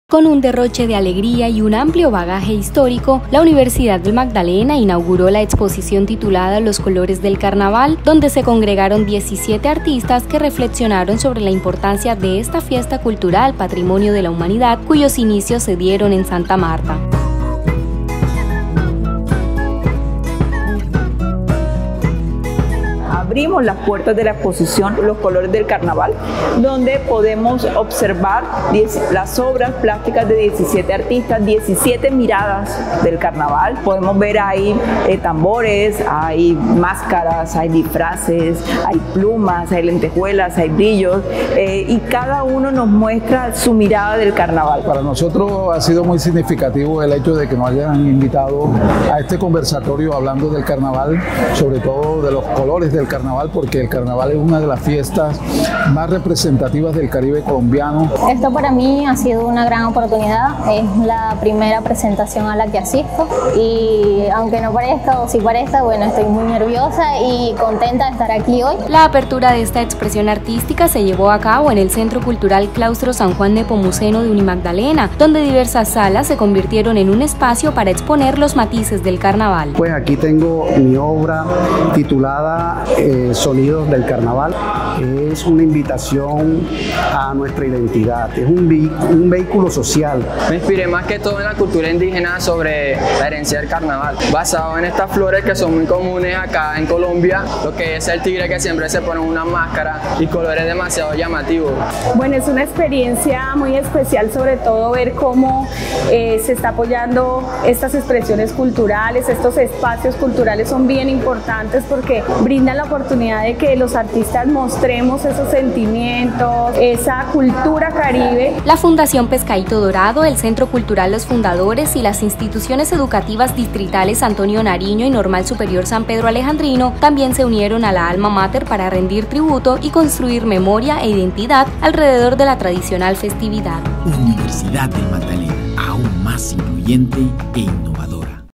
Con una mezcla de alegría e historia, UNIMAGDALENA inauguró exposición ‘Los Colores del Carnaval’
En el Claustro San Juan Nepomuceno, la Universidad del Magdalena dio apertura a la exposición denominada ‘Los Colores del Carnaval’, en la que se unieron 17 artistas que resaltaron la importancia de esta fiesta cultural patrimonio de la humanidad. Al derroche de alegría y abordaje histórico, se unieron fundaciones e instituciones educativas que también rindieron tributo y realizaron su aporte para la preservación del tradicional evento.